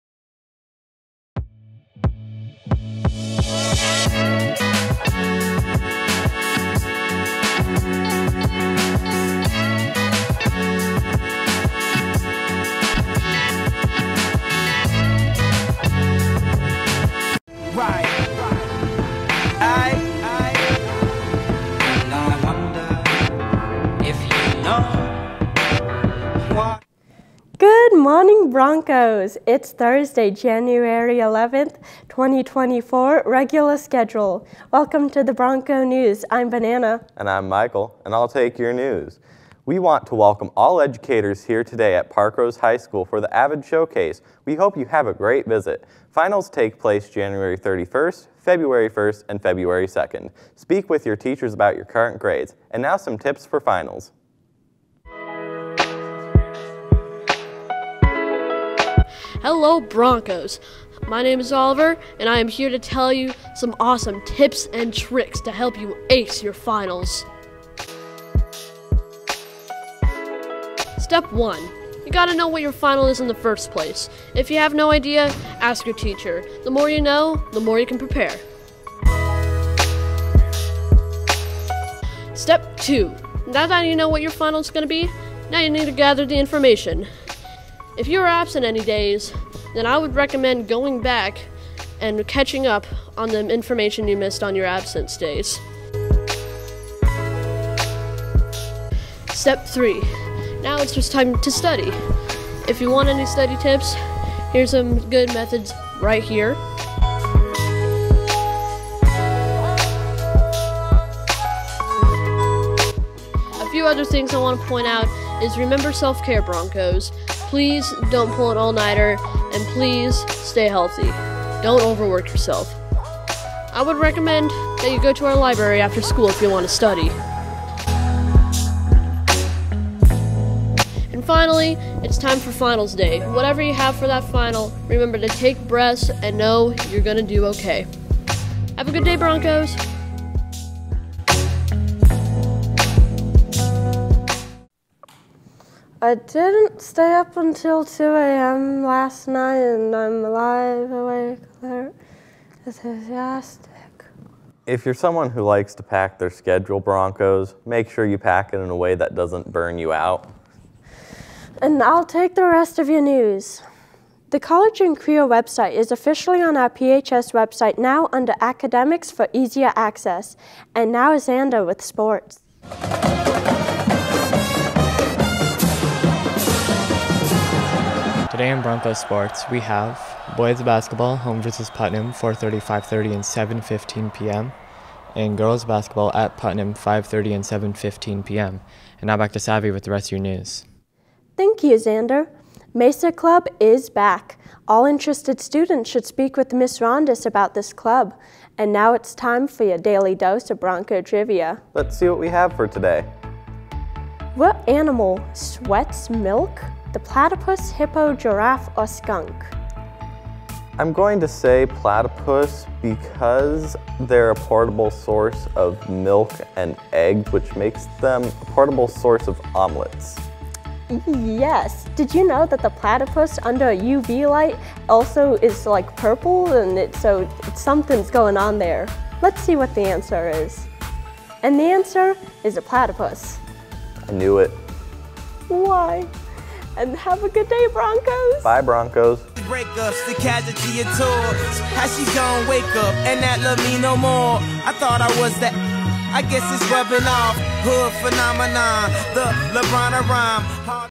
PCTV Bronco News 1.11.2024
Spot Video: What Are Your Parkrose Pet Peeves?
Today's show was produced by 2nd Period TV Newsroom Video Production Students!
Directed by Aneli Huerta